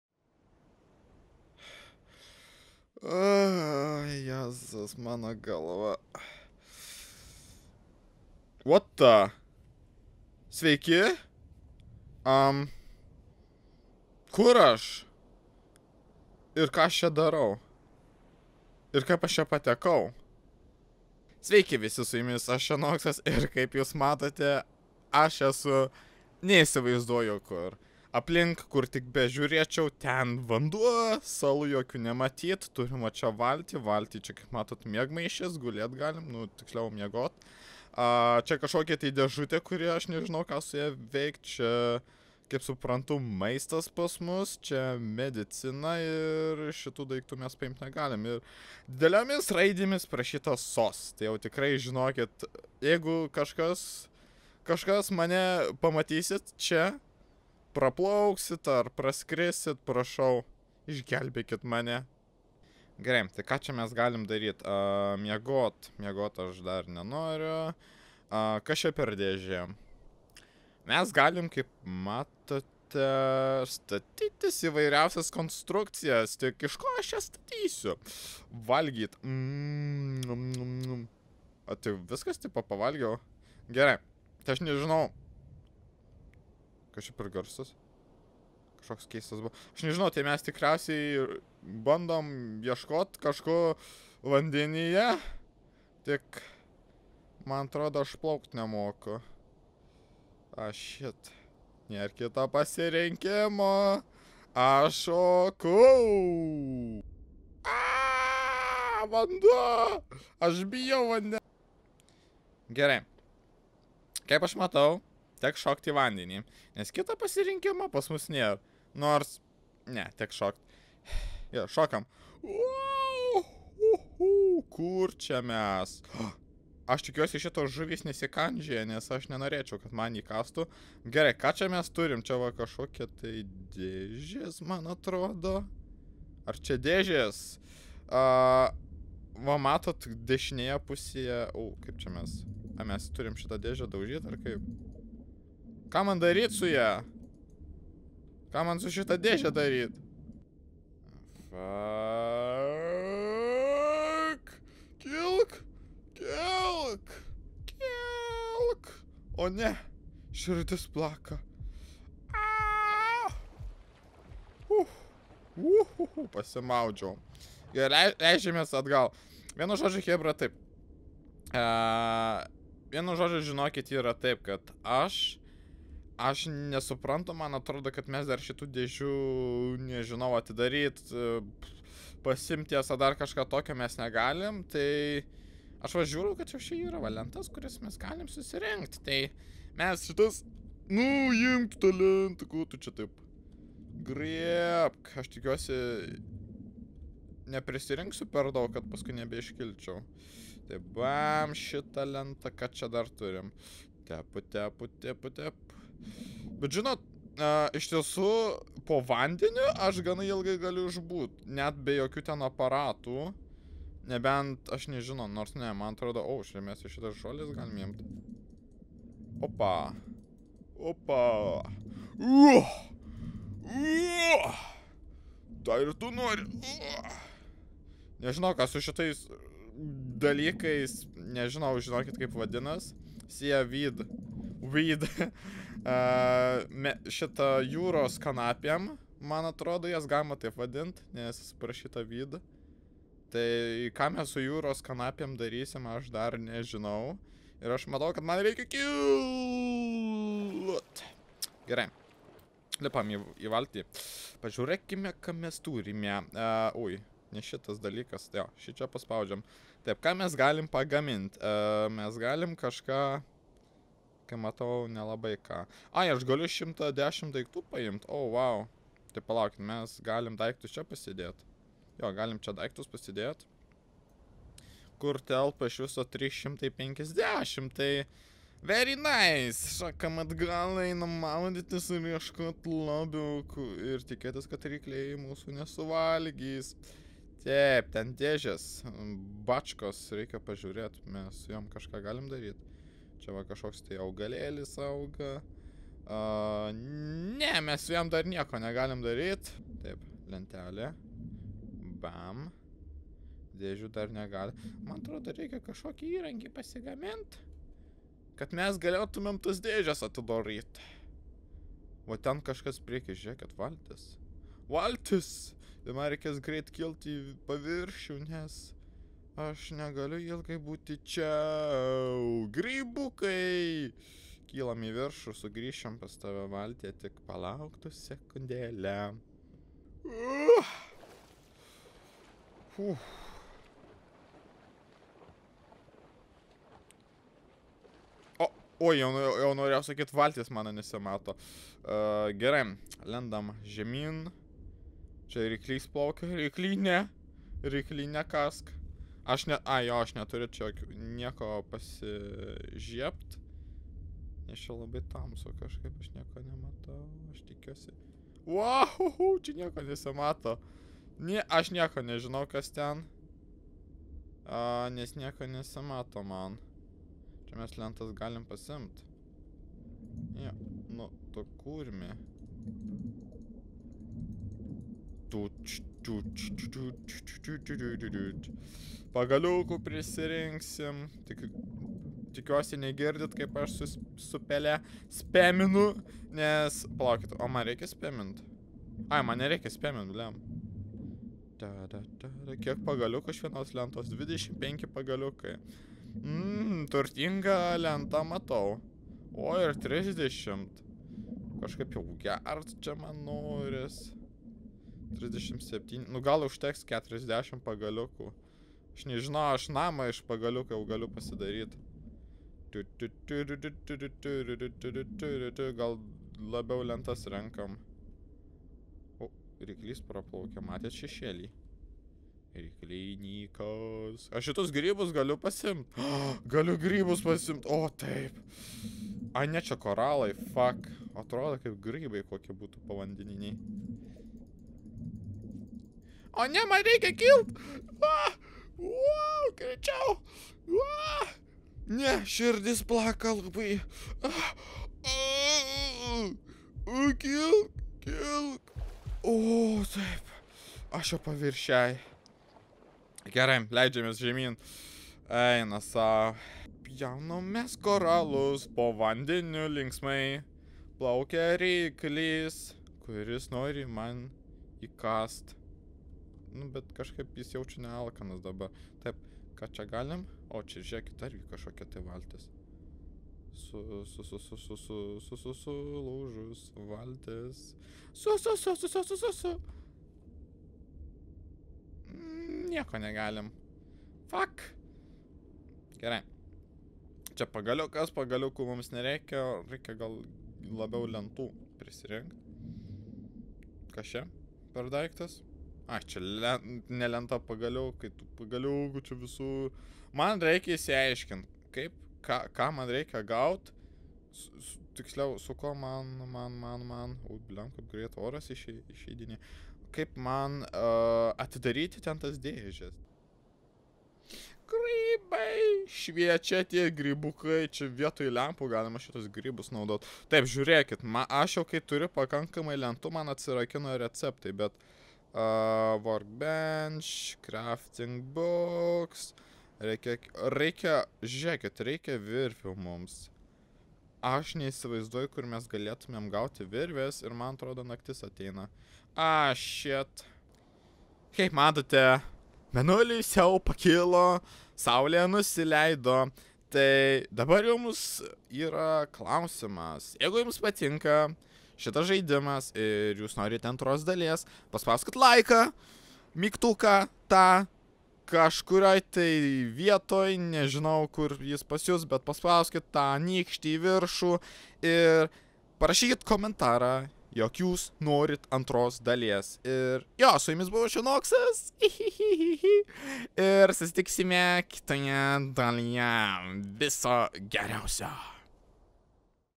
Paliktas rykliams
Dar vienas vaizdo įrašas! 🗻

Atsikėliau gelbėjimosi plauste , kuris yra viduryje kažkokio tai vandenyno... Kaip aš čia patekau?

Patiko? Spausk patinka mygtuką 💚
Nori daugiau? Spausk prenumeruoti mygtuką 💚
Nori kažką pasakyti? Palik komentarą 💚